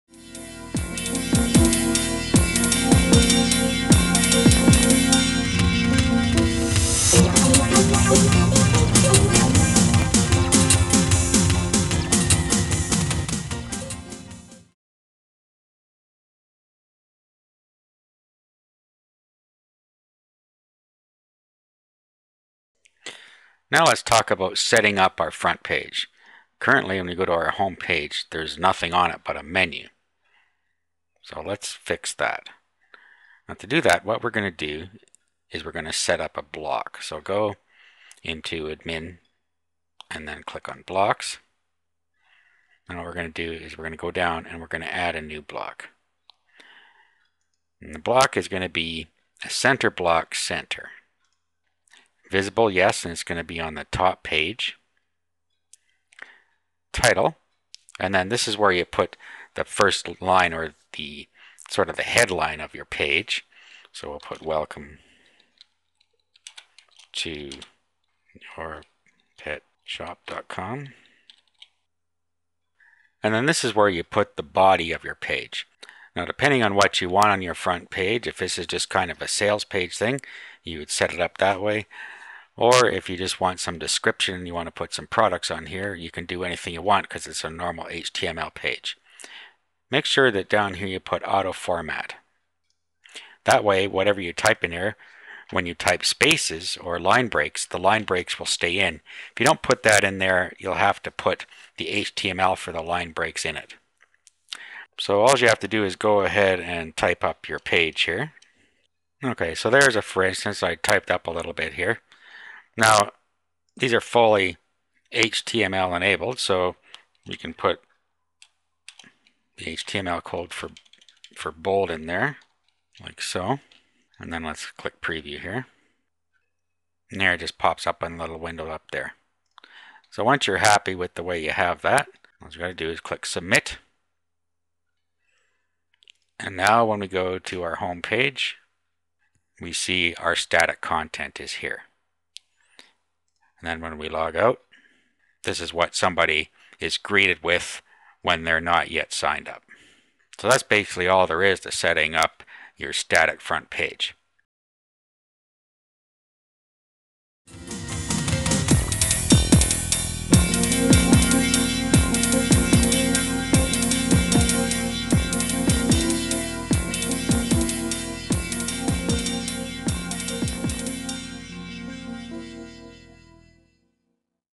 How to Setup Frontpage of Xoops
It's time to fill in some content on the blank frontpage.